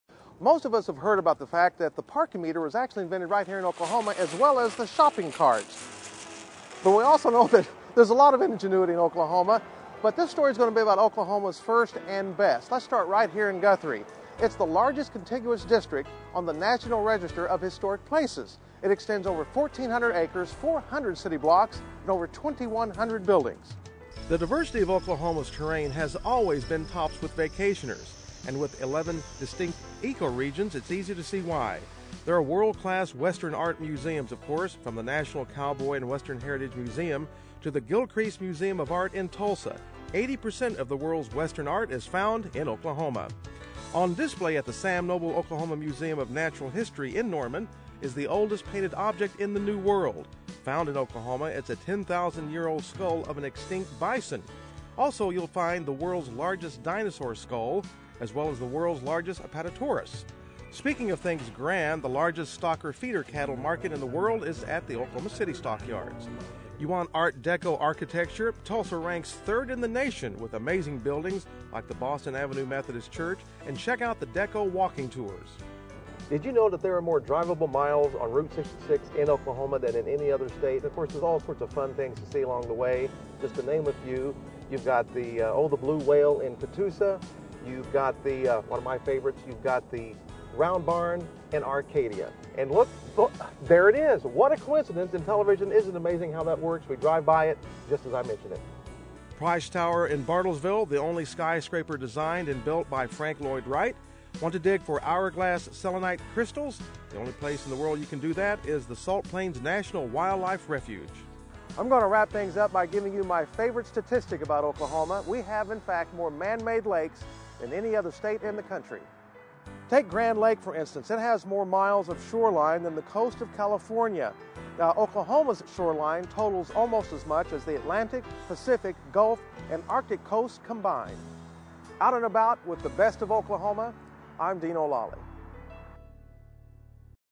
Okie Inventions and Lots of Other Great Things About Oklahoma
If you've lived in Oklahoma for a time, you most certainly have heard that the parking meter (Ca-ching!) and the shopping cart were invented here, so you know that there is a lot of Okie ingenuity going on. But our Dino Lalli likes to point out other features that make Oklahoma such a great place to live... and an outstanding place to visit.